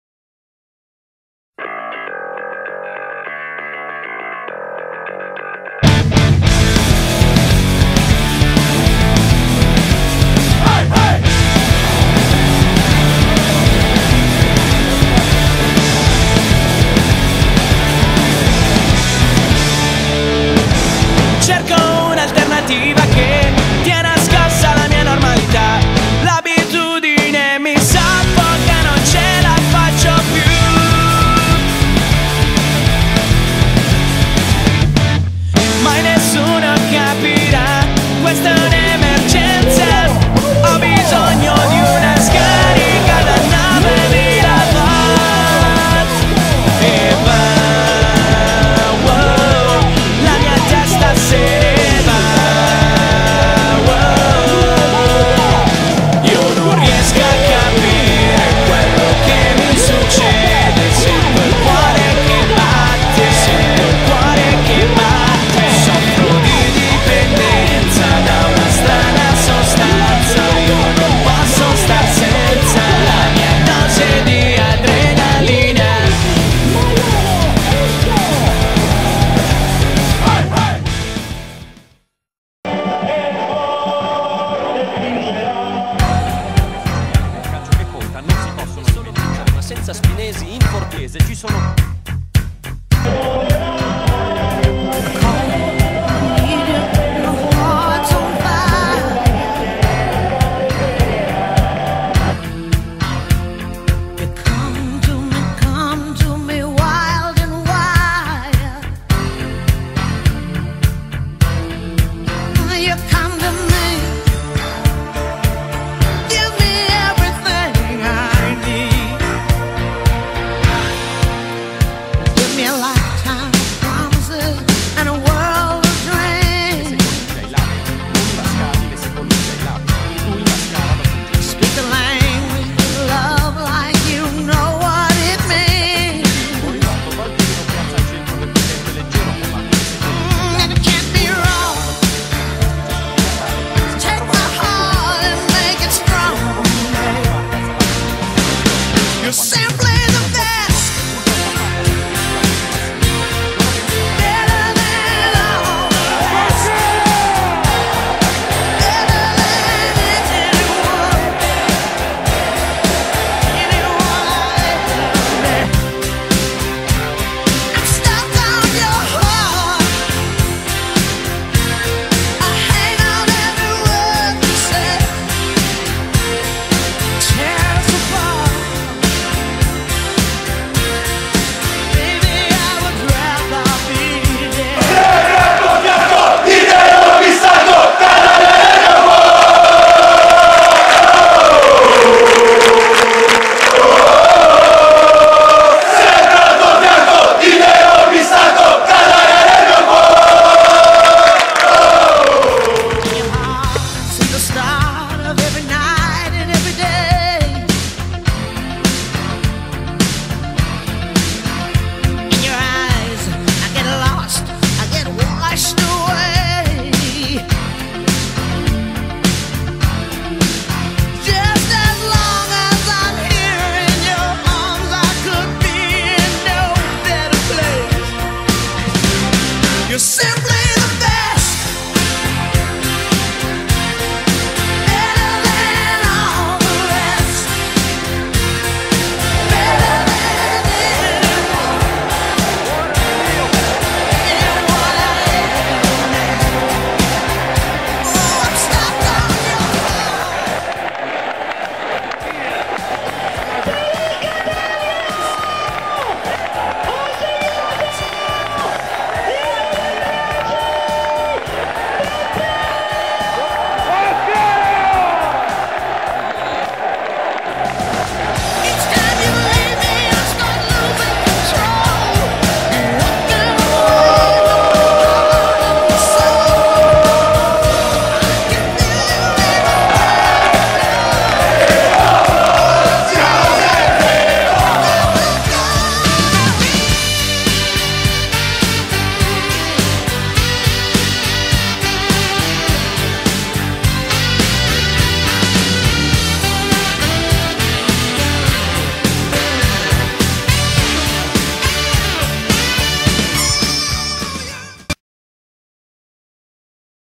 Catania Udinese IL FILM
Una doppietta di Peppe Mascara permette al Catania di superare l'Udinese del grande ex Pasquale Marino, nell'anticipo pomeridiano della 16a giornata. Il tutto in un film firmato Mondocatania.com